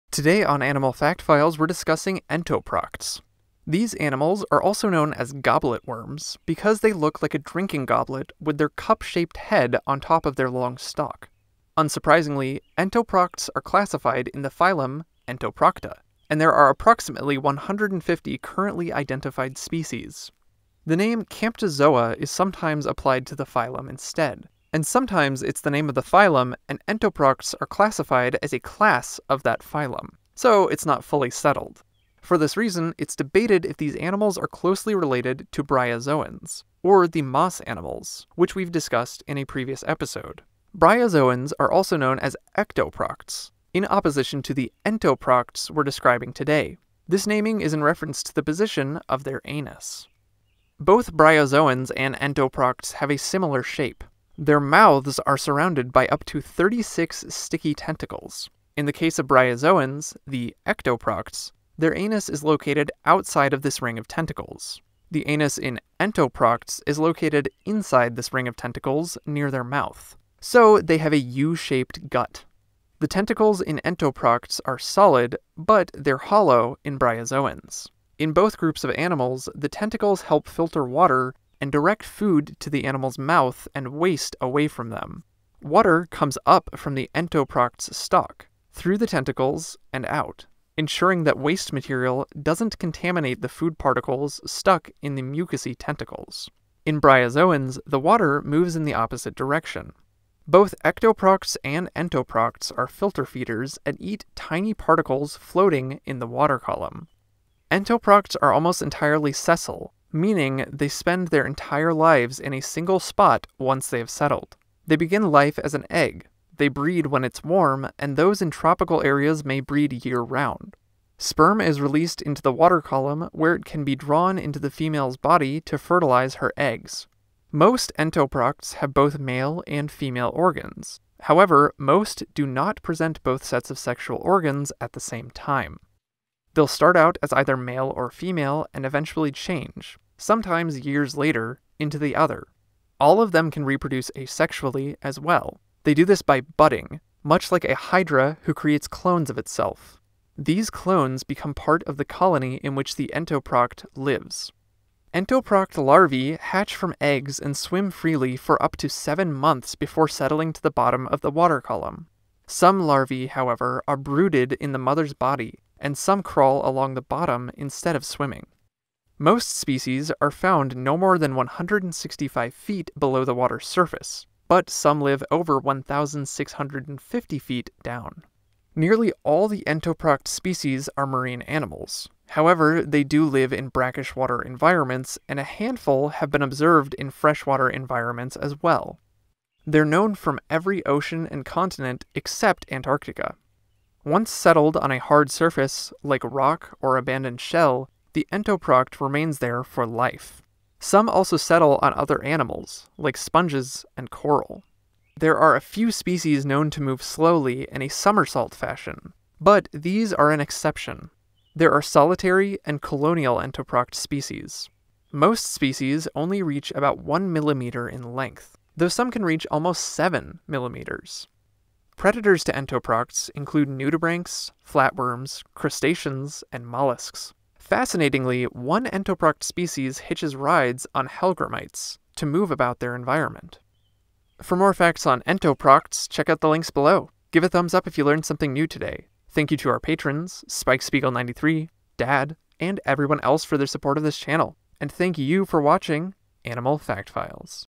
Entoprocta Facts: an ANIMAL You've NEVER HEARD OF | Animal Fact FIles
Entoprocts are probably an animal you've never heard of. They are typically classified in the phylum Entoprocta, but even that's debated. Sometimes they're known as Kamptozoans! Learn more Entoprocta facts on this episode of Animal Fact Files!

Scientific Name: Phylum - Entoprotca
Range: worldwide except Antarctica
Habitat: marine coasts, brackish estuaries, freshwater
Size: less than a centimeter
Diet: filter feeding - detritus
Threats: nudibranchs, flatworms, crustaceans, and mollusks
Lifespan: at least seven months

What is an Entoproct? 0:00
Entoproct APPEARANCE 0:49
Entoproct LIFE CYCLE 1:46
Entoproct HABITAT and RANGE 2:45
Entoproct SIZE 3:31
Thanks / Outro 3:52